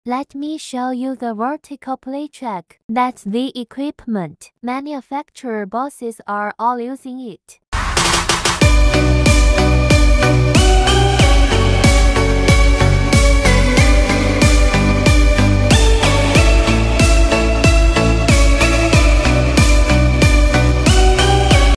vertical sheet storage mould storage drawer structure storage rack
🔹specificities :
- Offers an efficient solution for storing metal sheets and remnants.
- Provides a clear overview and easy access to all materials.
- Designed for compact storage, maximizing space efficiency.
- Features pull-out drawers for direct access to metal plates.
- Allows quick access to desired materials, significantly saving time.
- Reduces operational costs by improving storage efficiency.